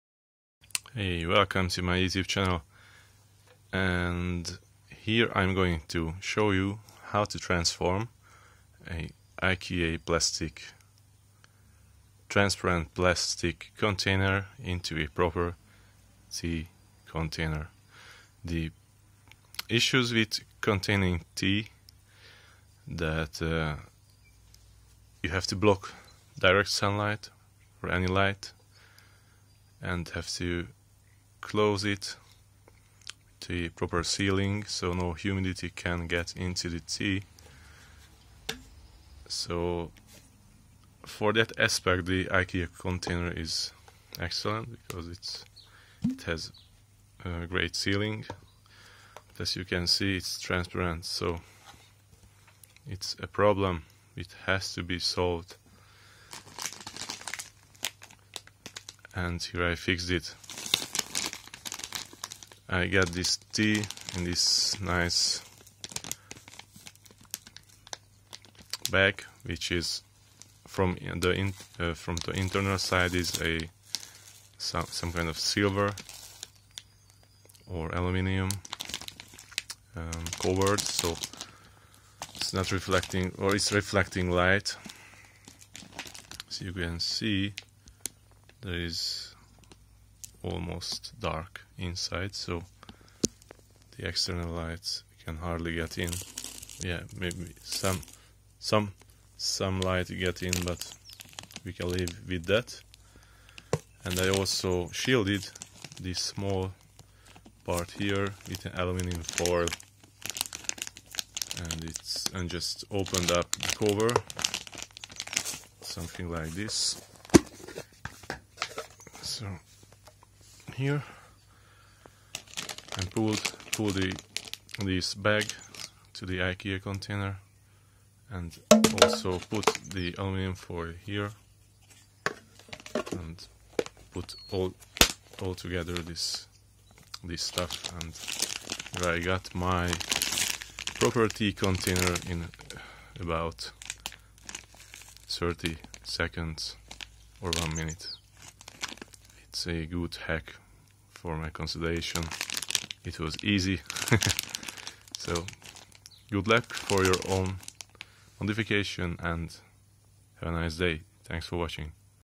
DIY Tea Storage from cheap transparent IKEA plastic box
For proper tea storage you have to block humidity and sunlight. Transparent IKEA boxes only solving the first problem, but not blocking sunlight. So I've used the tea plastic bag itself for light shielding. Works perfect! :)